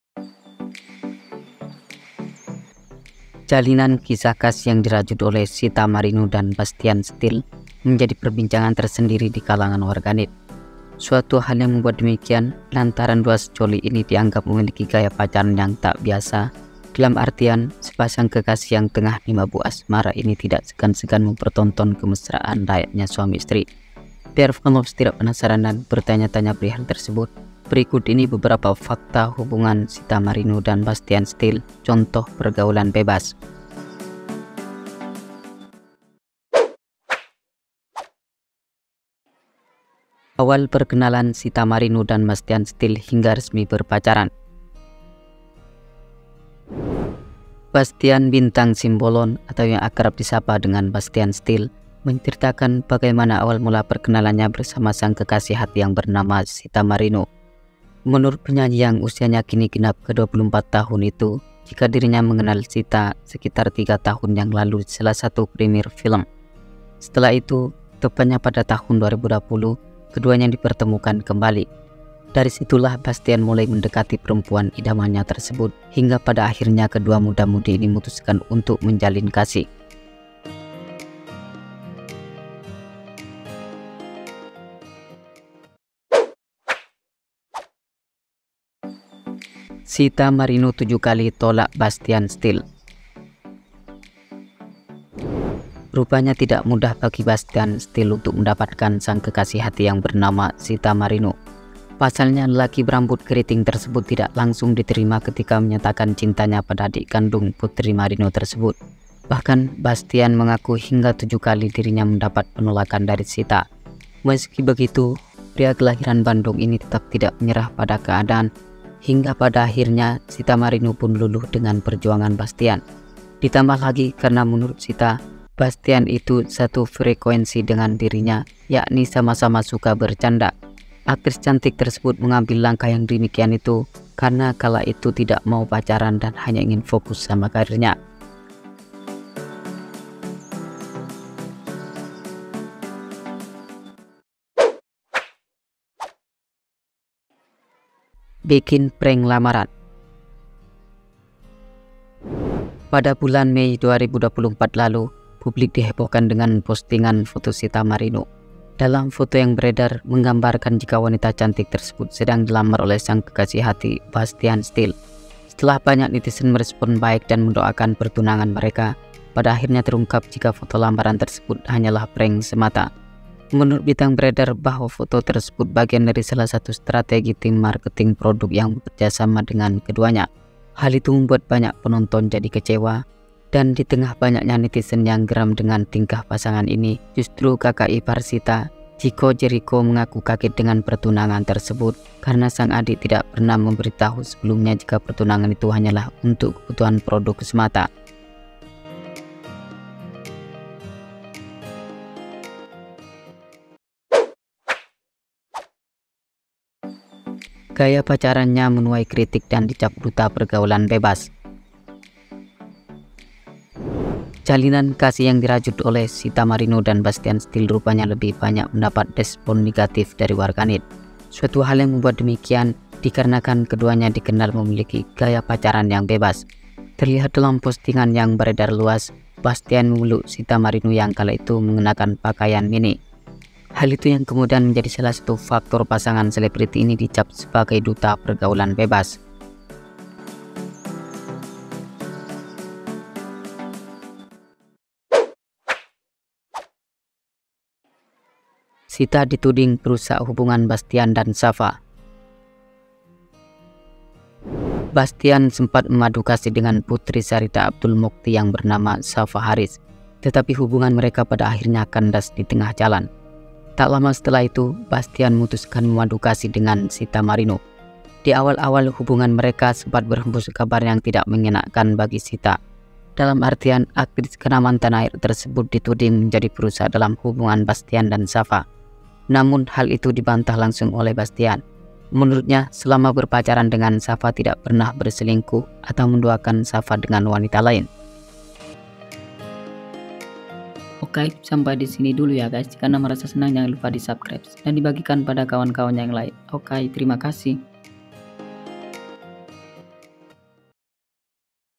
Fakta Hubungan Sitha Marino dan Bastian Steel, Contoh Pergaulan Bebas?
Jalinan kasih yang dirajut oleh Sitha Marino dan Bastian Steel menjadi perbincangan yang aktual di kalangan warganet.
Suatu hal yang membuat demikian karena pasangan yang tengah dimabuk asmara ini dikenal memiliki gaya pacaran yang terbilang bebas.

Berikut ini adalah beberapa fakta hubungan Sitha Marino dan Bastian Steel, contoh pergaulan bebas?